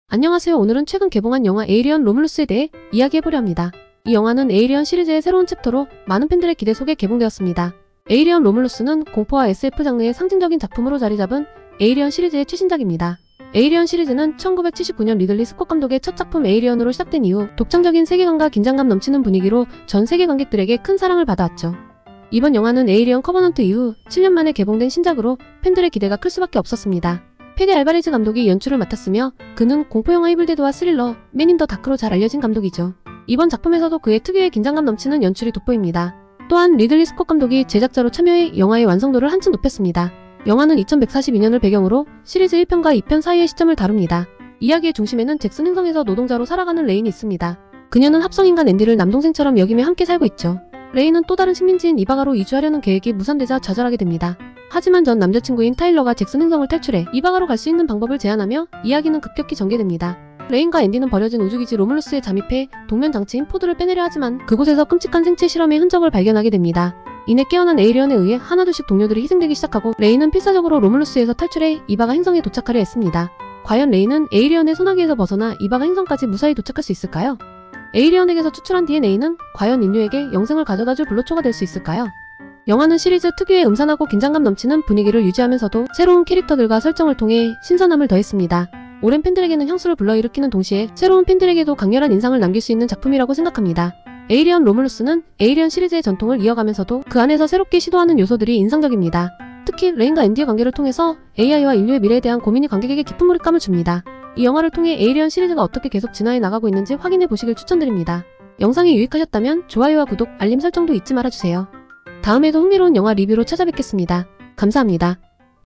순한 맛이지만 강렬한 "에이리언: 로물루스" / 1편과 2편 사이의 이야기 / 새로운 젊은 캐릭터들 / 강렬한 긴장감과 몰입감
영화직관하는남자 홍큐의 "에이리언: 로물루스" 후기입니다.
*쿠키영상은 없어요.